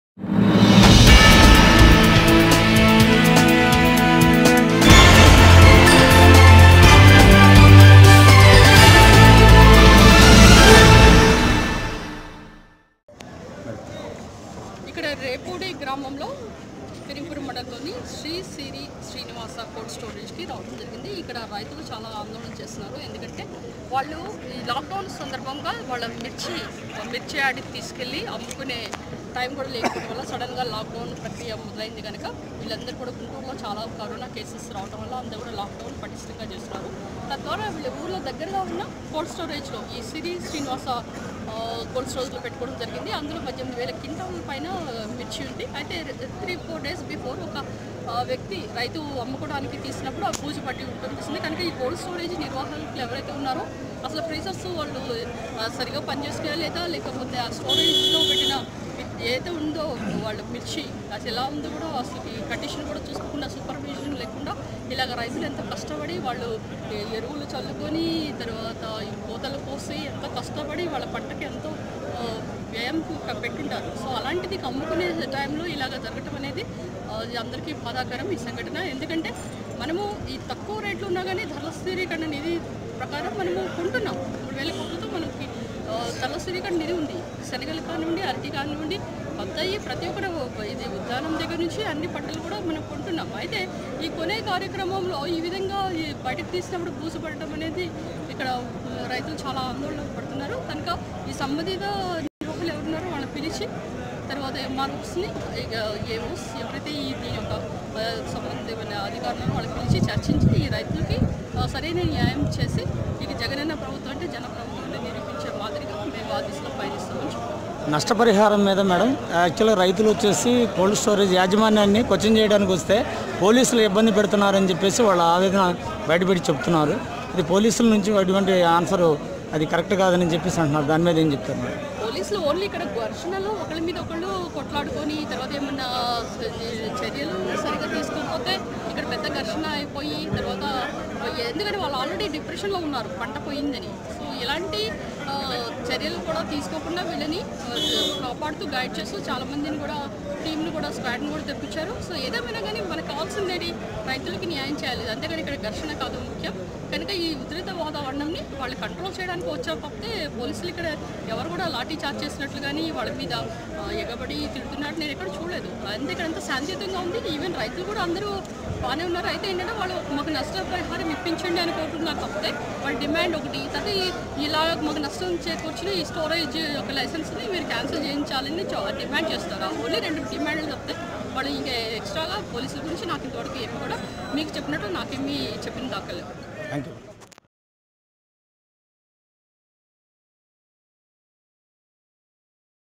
రేపూడి గ్రామంలో కోల్డ్ స్టోరేజ్ సందర్శించిన తాడికొండ గౌరవ ఎమ్మెల్యే శ్రీ ఉండవల్లి శ్రీదేవి....
తాడికొండ నియోజకవర్గం, గుంటూరు జిల్లా...